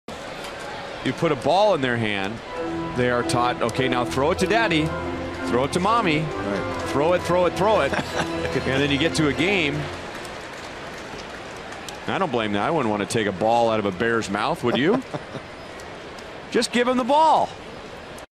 2013/06/29 T.C. Bear scares a child
KC@MIN: T.C. Bear scares a small child in the stands